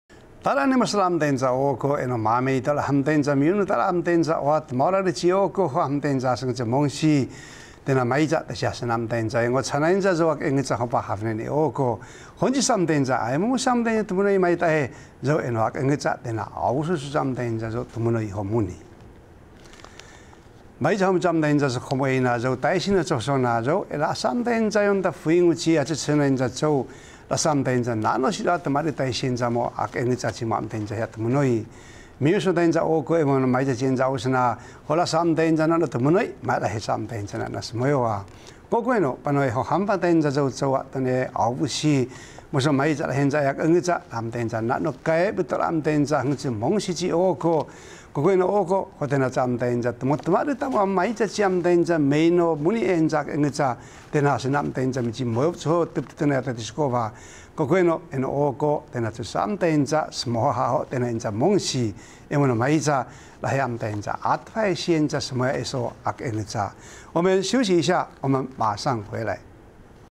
轟隆!雷公生氣了 長輩嚇阻童哭鬧 2015-08-08 Cou TITV 原視族語新聞
titho ta ak'e ng ʉ ca poa tosvo monsi 'e o'oko

記得小時候，長輩們對著小朋友要聽話，不要哭鬧，要不然雷公會把你帶走，如果你哭鬧不停，雷聲會愈大聲==NS==夏季有典型午後雷陣雨，而在山區生活的原住民，最能感受雷雨交加的情形，對於小孩子來說如此轟隆作響，實在恐怖極了，而在鄒族裡有關雷公的傳說，話說雷公會抓走哭鬧的小孩，小孩一聽到大人說雷公要下凡抓人了，小孩們就會安安靜靜的不出聲，因為他們最怕雷公「ak'e ngʉca」。

tmunoi co ak'engʉca
雷公叫的好大聲

ote aoko monsi
      tasi hafa ta ak'engʉca
不要哭了雷公會把你帶走